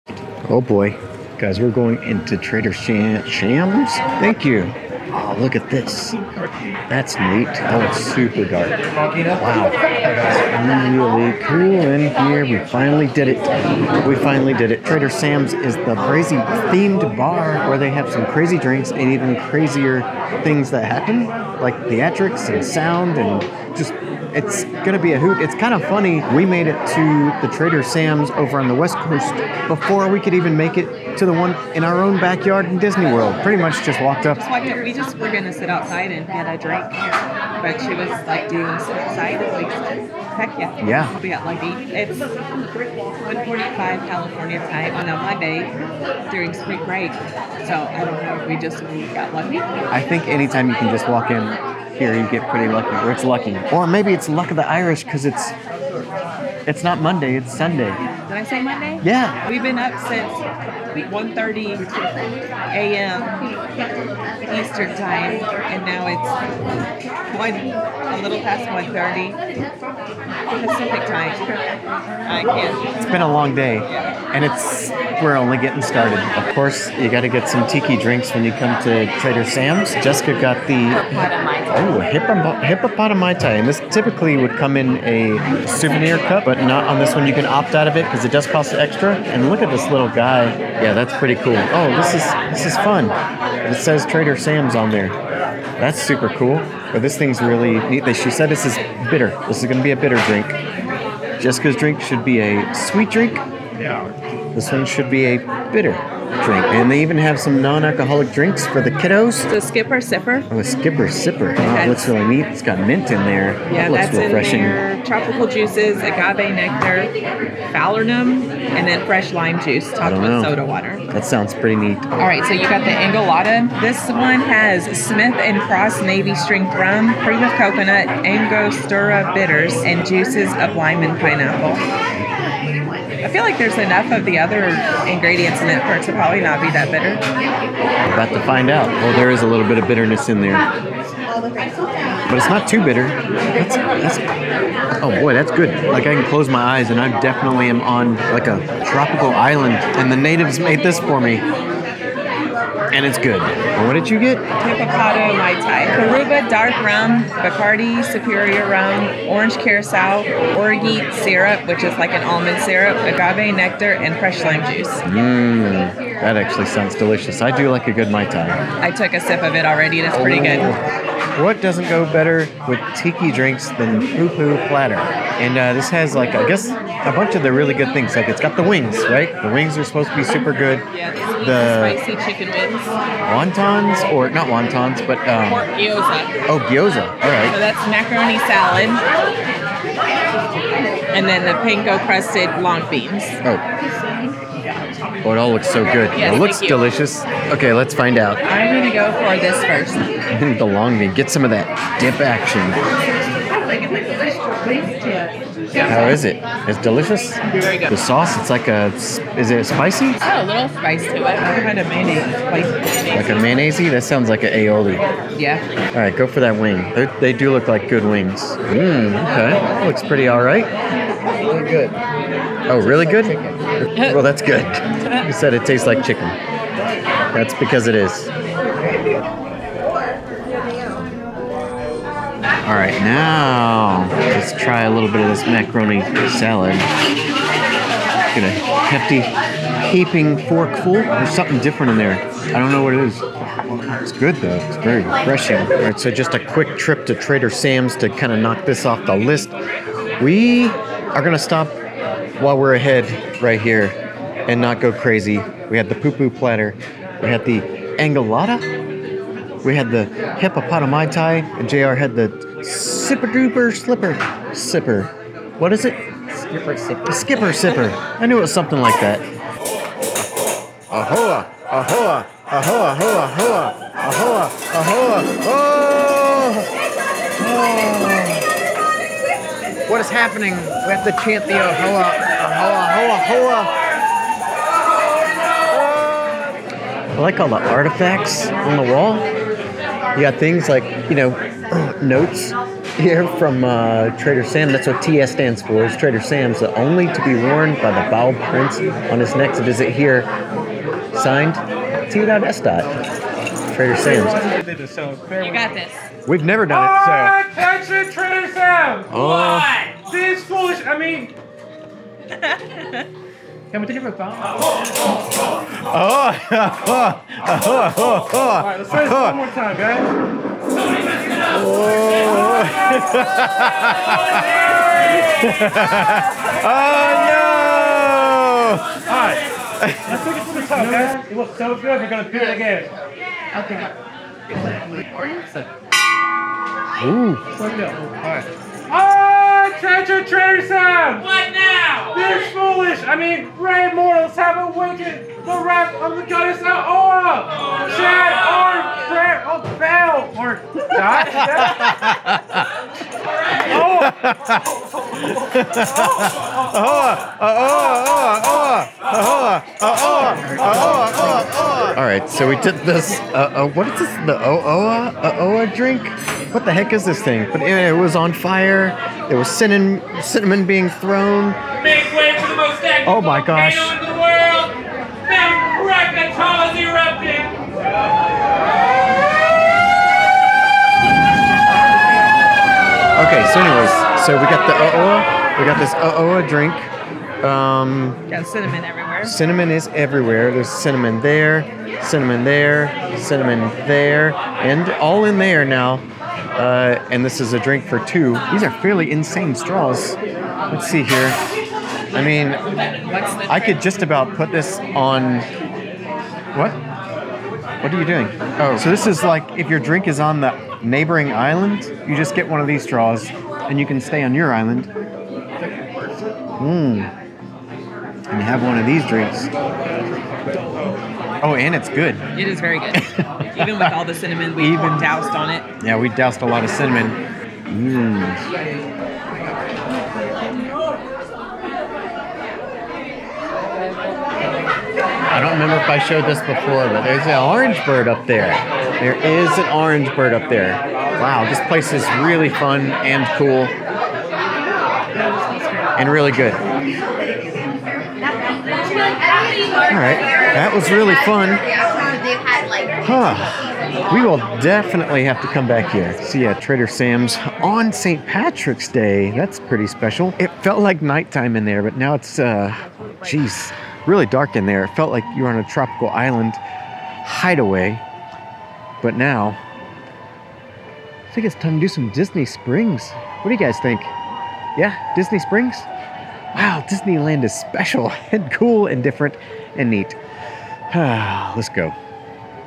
Incredibly Magical Trader Sam's in Disneyland
Embark on a whimsical journey to the heart of Disneyland's enchantment with our first visit ti Trader Sam's Enchanted Tiki Bar. Join us as we step into this hidden gem, where the spirit of adventure and the magic of Disney come alive in every sip.